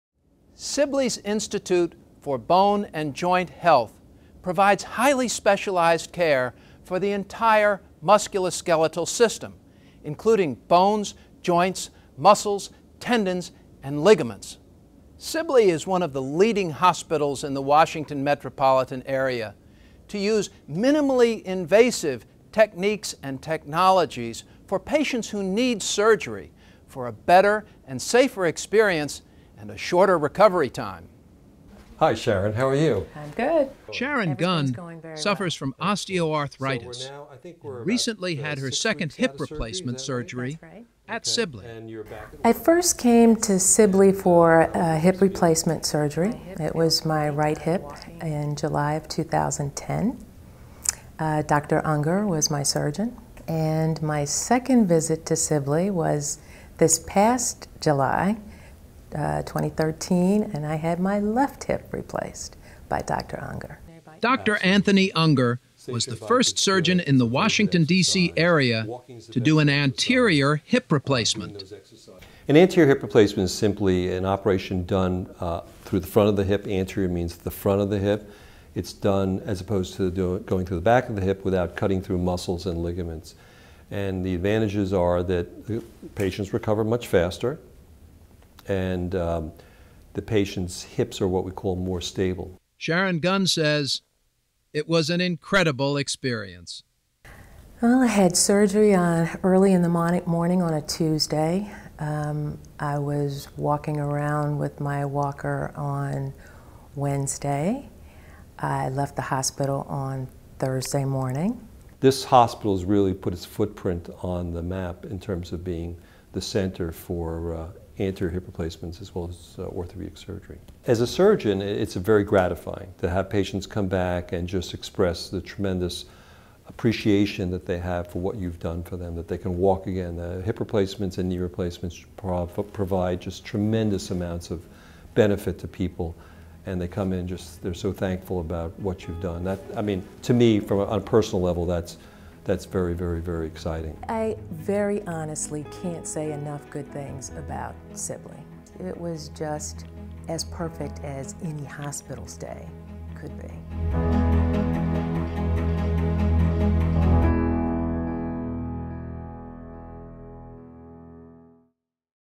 Sibley Memorial Hospital - Institute for Bone and Joint Health
Learn more about Sibley's Institute for Bone and Joint Health with Anthony Unger, M.D., Orthopedic Surgeon, Institute for Bone and Joint Health.